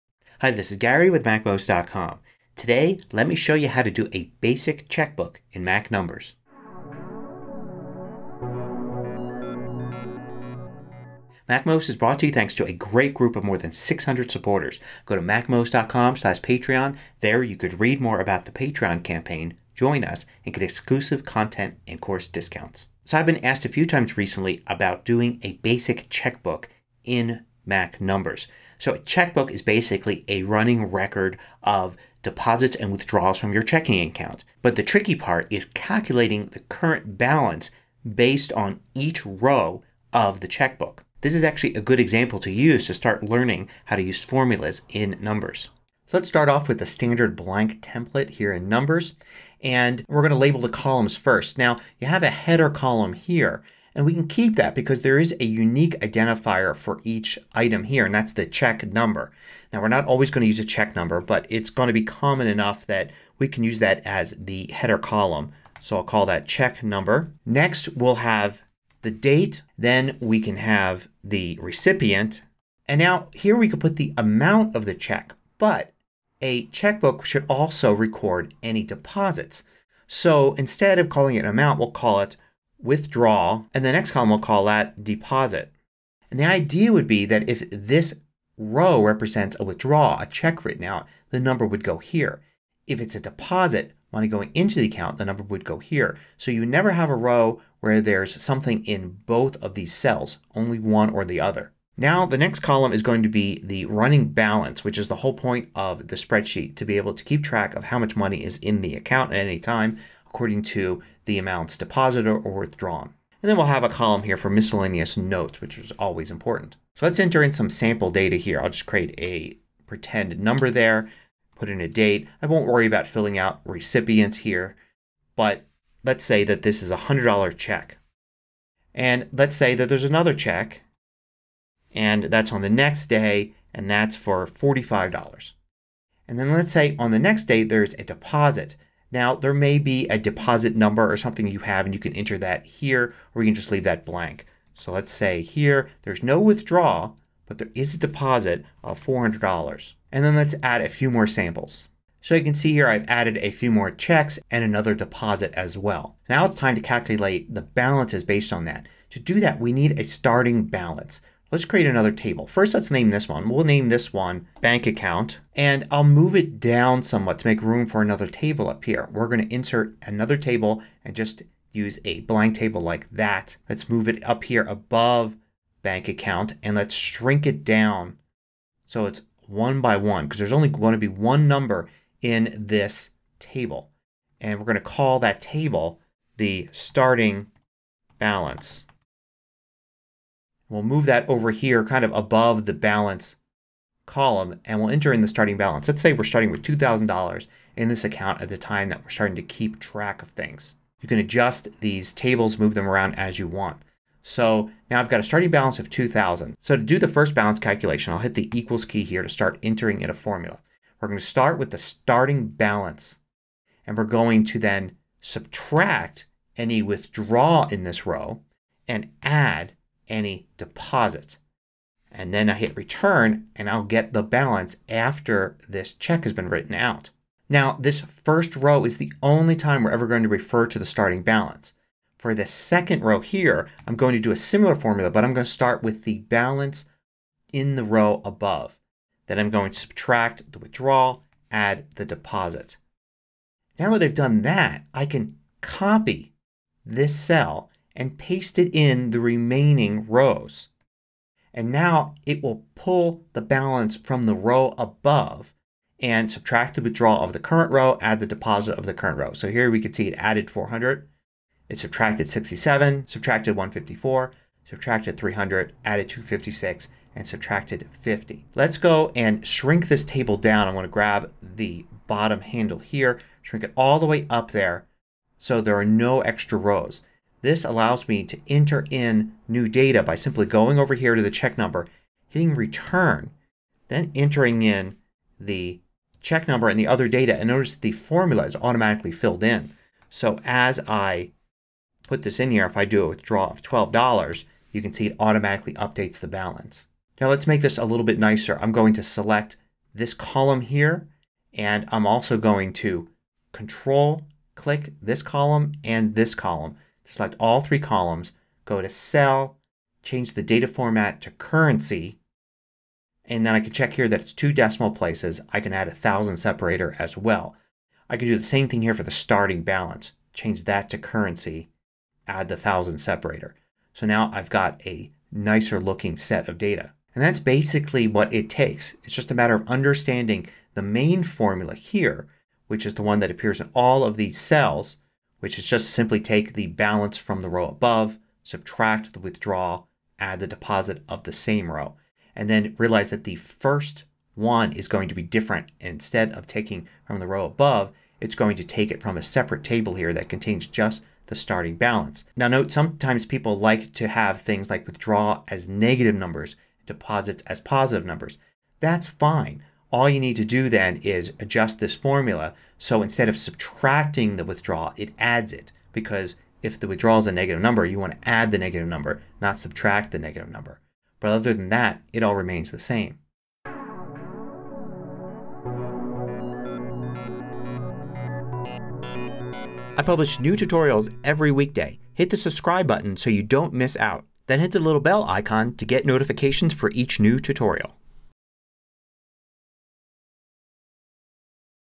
How To Make a Basic Checkbook Spreadsheet With Numbers On a Mac
A good way to learn some basic Numbers skills is to build a simple checkbook. You can use this to keep track of your bank account balance.